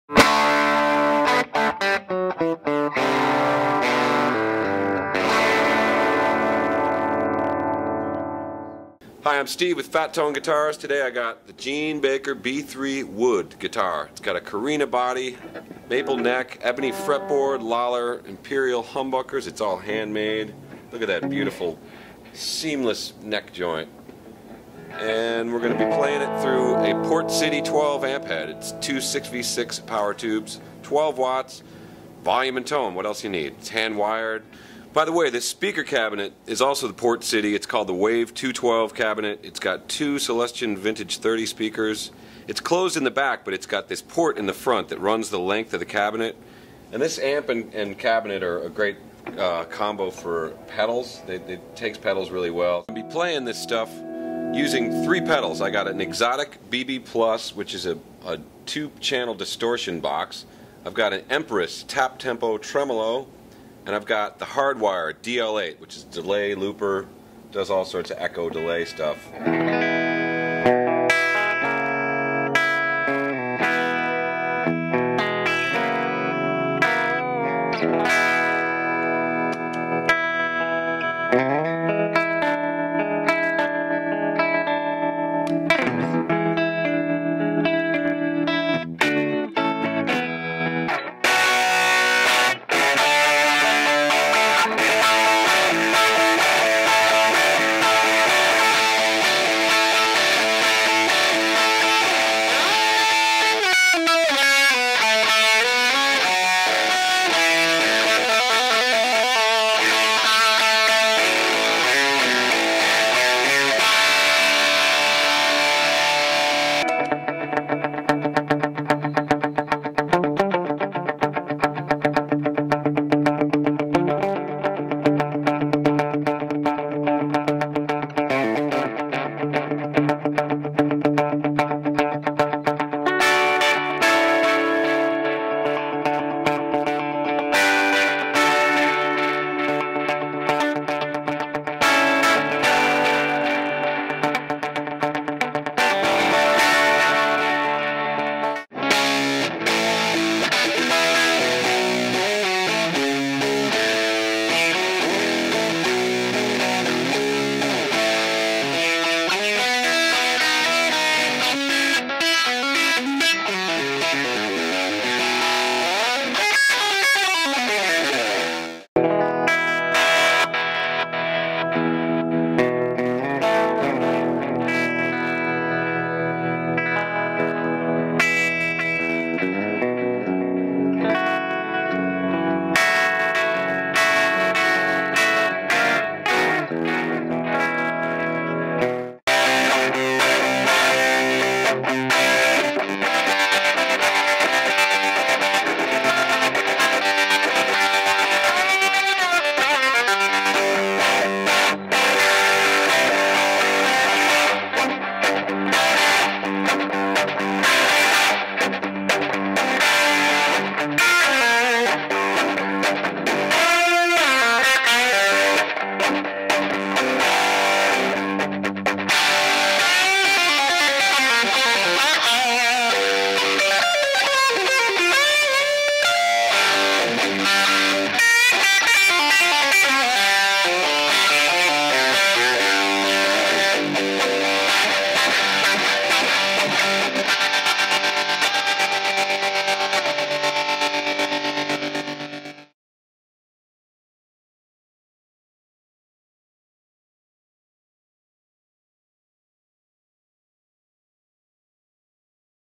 Fat Tone Guitars - Port City Wave and Gene Baker B3 Guitar
Steve Gerlach gets it done with the Gene Baker B3 Wood Guitar through the Port City Twelve Amp and Wave 2x12 Cabinet.